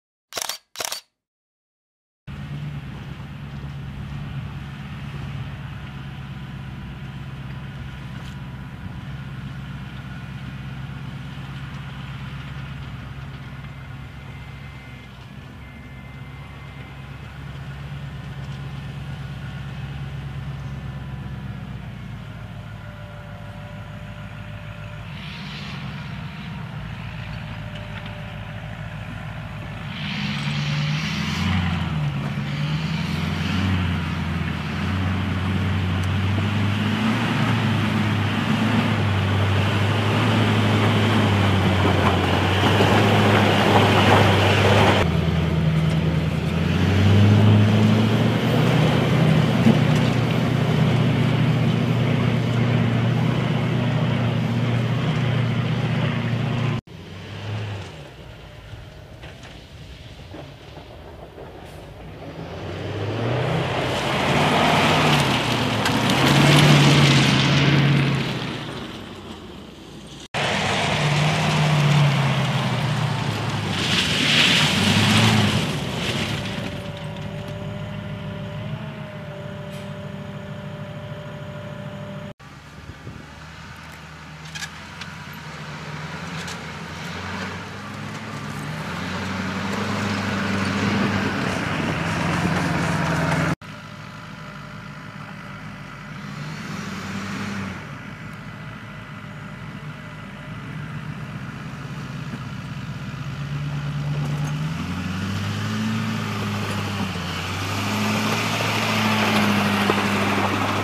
Joint Light Tactical Vehicle (JLTV)
Imágenes de una demostración de capacidades del Joint Light Tactical Vehicle (JLTV), el sustituto del Humvee como vehículo multipropósito de las Fuerzas Armadas de Estados Unidos, en la base de los Marines en Quantico (Virginia).

Vídeo original: DVIDShub.net.